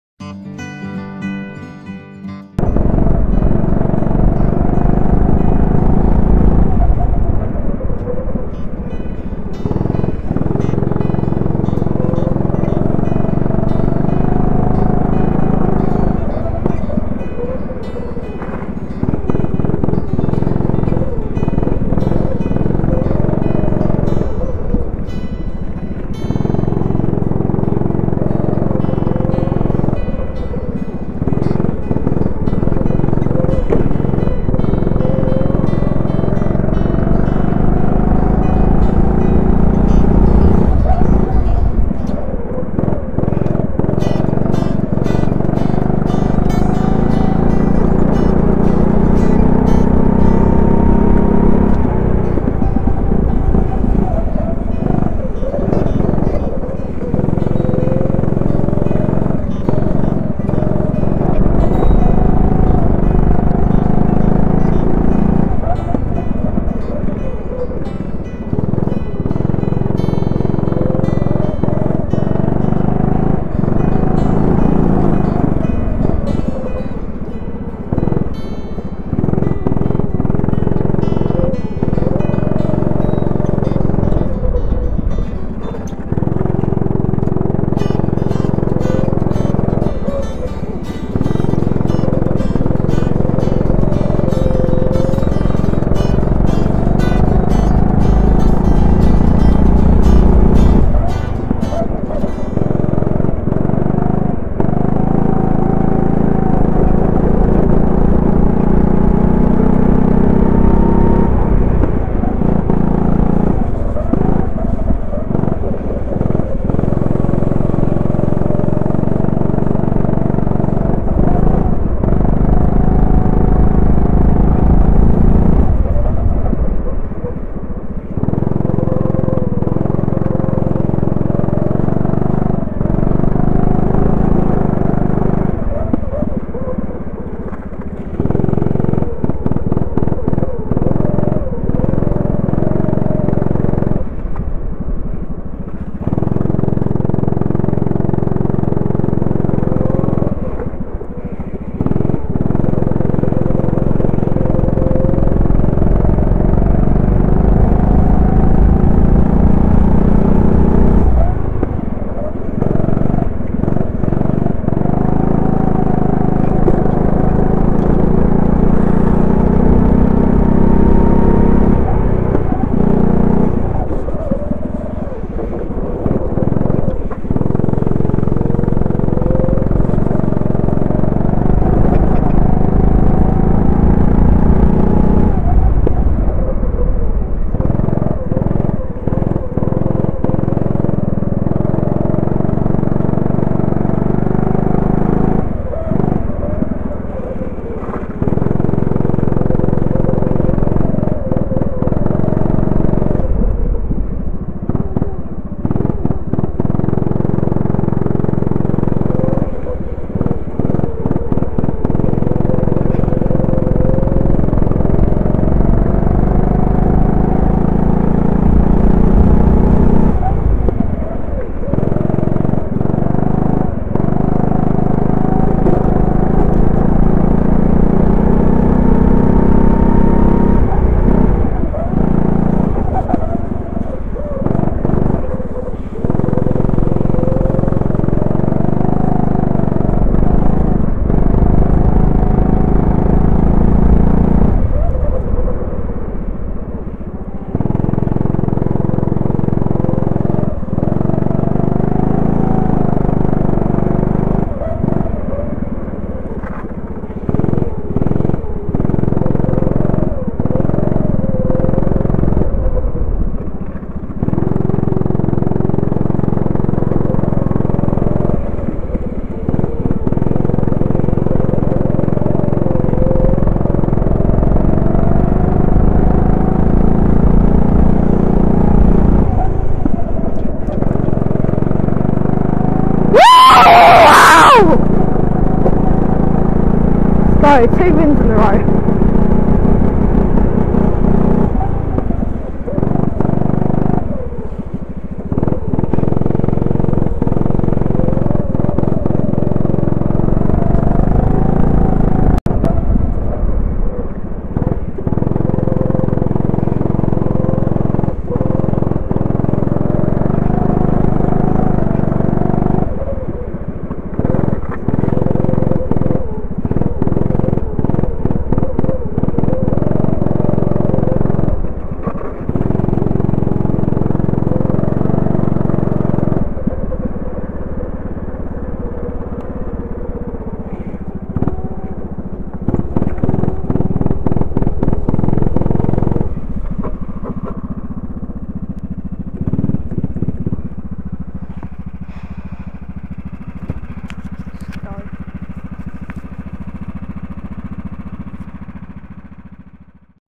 Bayford Meadows - Junior Club Round 9 - 05.11.22 - Heat 2
The track was wet, which is my favourite type of track - so won heats 1-3 and come 2nd in heat 4 was having fun drifting and run out of talent, but the win on the day - clinched my 1st place in the in the championship : )

Thanks to @SoarEKarting @Sianformworks @Spitfireboxing @H&O @socompliant @JBEmbroidery @bayfordmeadows for your support.